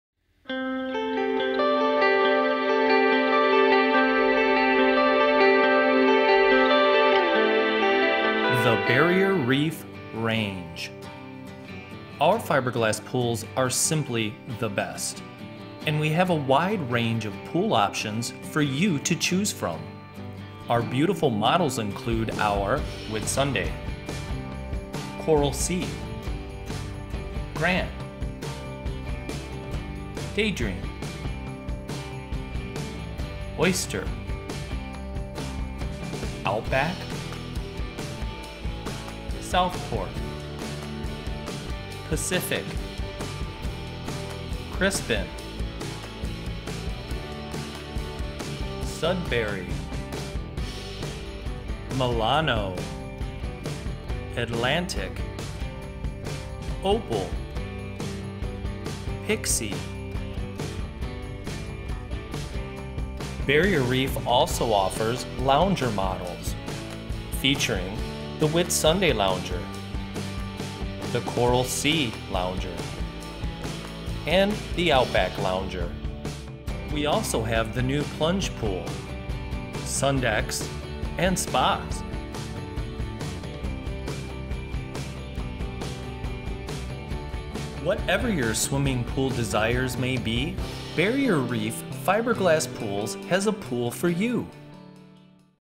Fiberglass Swimming Pool Models | Barrier Reef Fiberglass Pools
Have a look at our fiberglass swimming pool models. Barrier Reef fiberglass swimming pools are simply the best. Get into a fiberglass pool today!